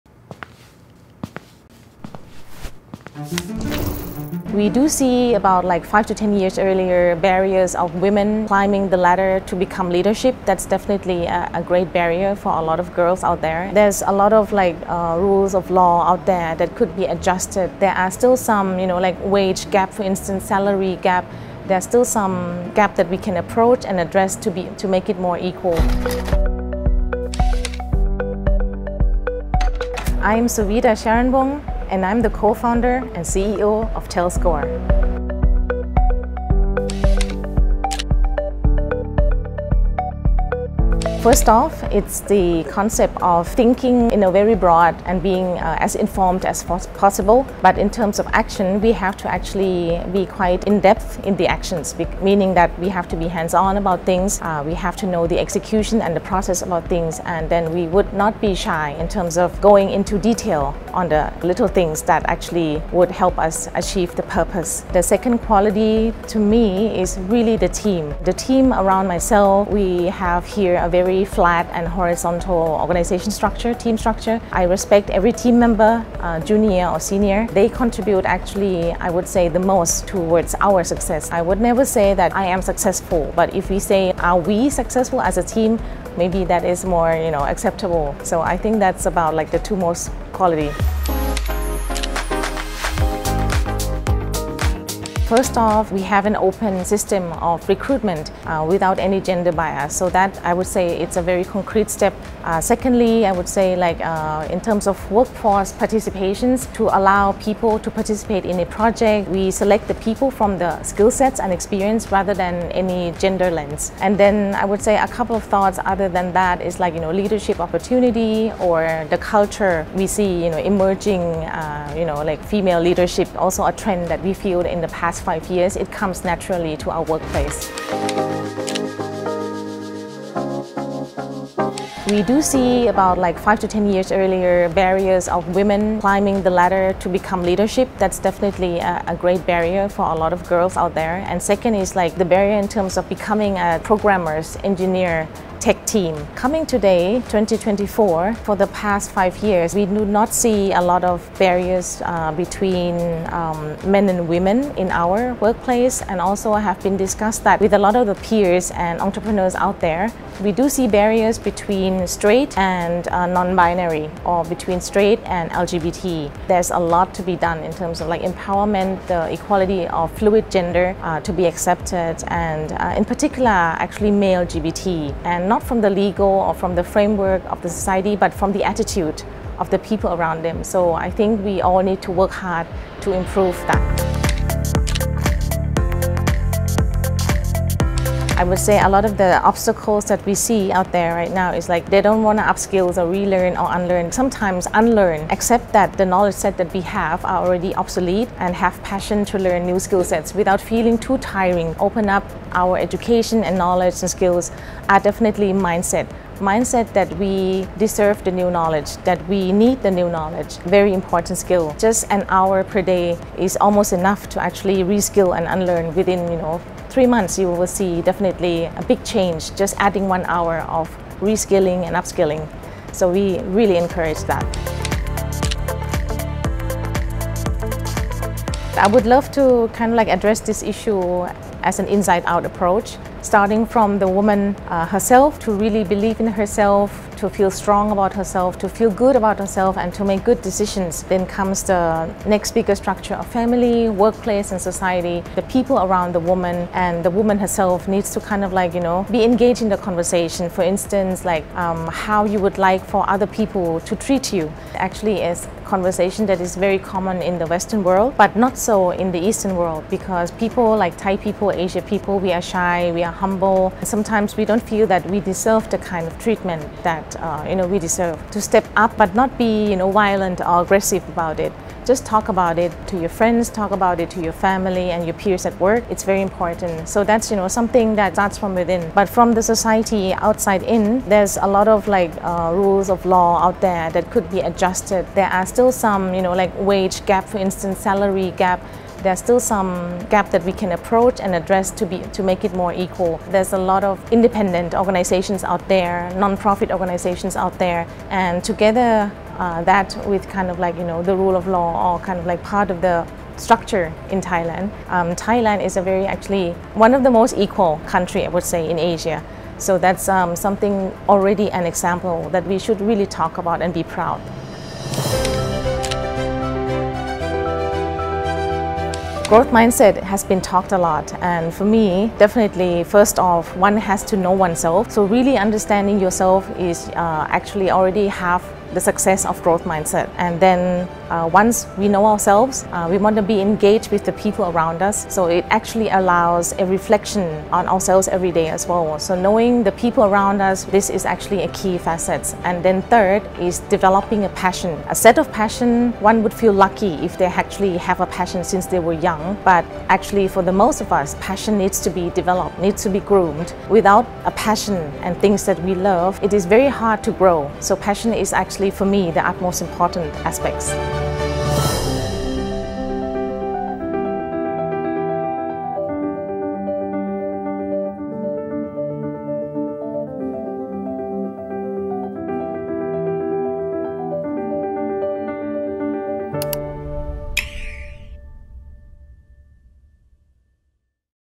The advocate of equality in the workplace: Suvita Charanwong, CEO and co-founder of Tellscore.
Despite the emerging trend of more women CEOs than ever before, still women have fewer numbers of C-level executives than men.
The Nation exclusively interviewed Suvita Charanwong, CEO and co-founder of Tellscore, a leading MarTech company, a businesswoman who has a voluntary vision and advocates the equality in workplace.